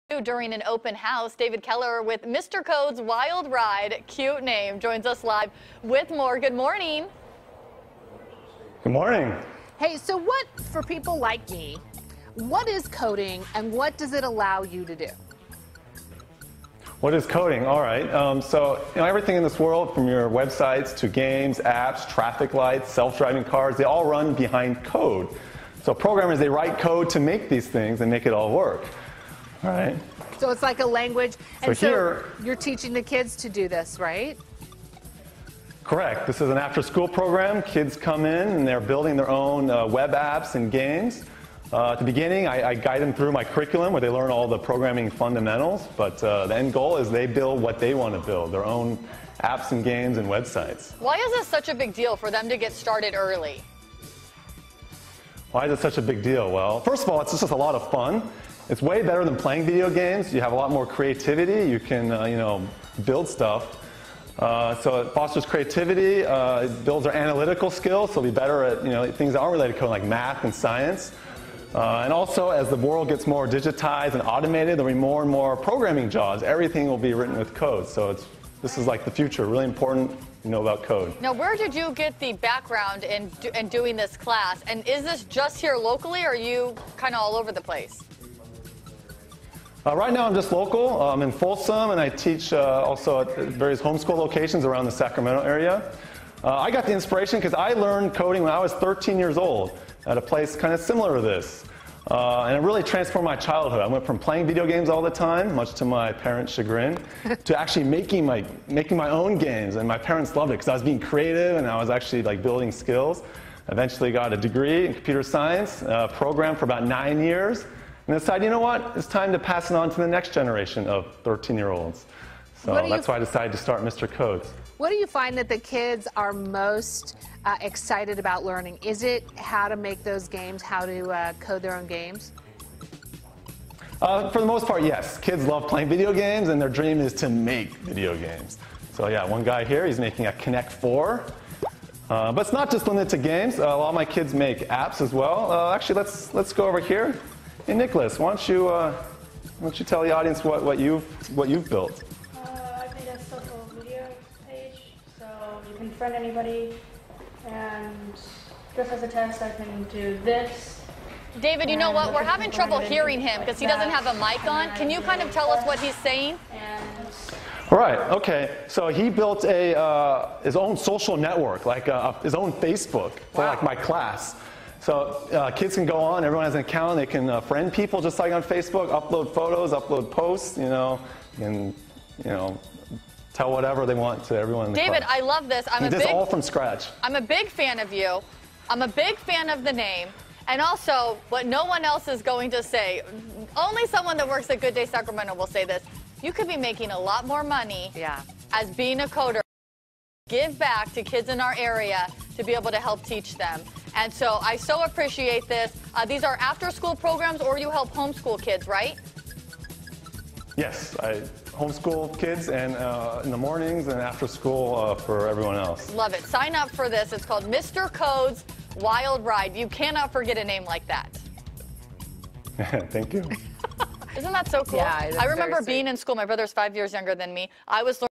Coding Class Open House
Mr. Code's Wild Ride is hosting an open house to the public where anybody can check out coding classes for kids (Ages 10-18).